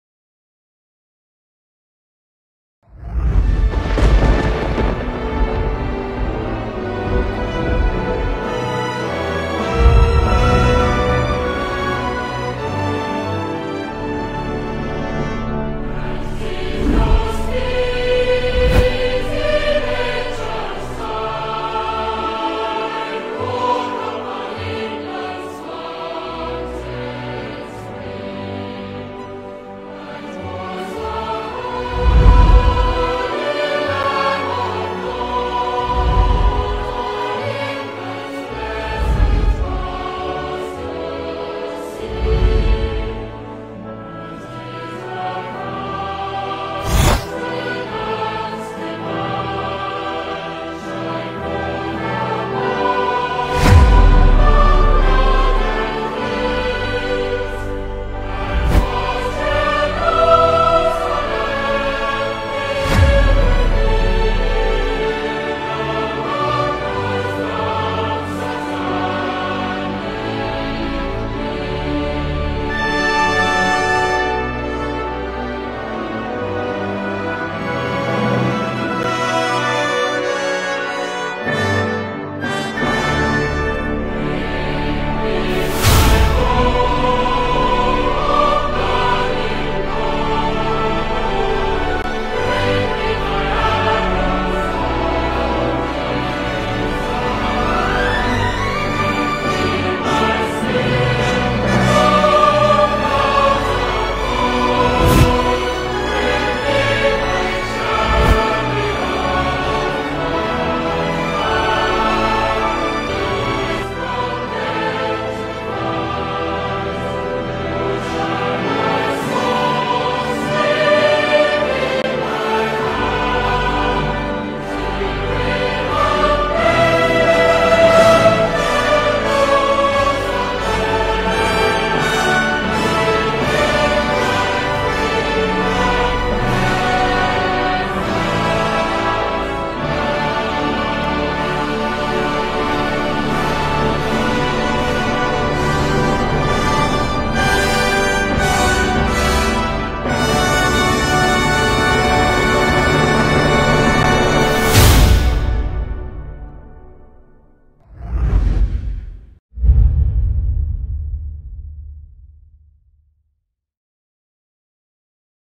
England from the sky 2023 | 4 K
Yorkshire, Kent, Hampshire, Surrey Hills, West Sussex, Lake District and more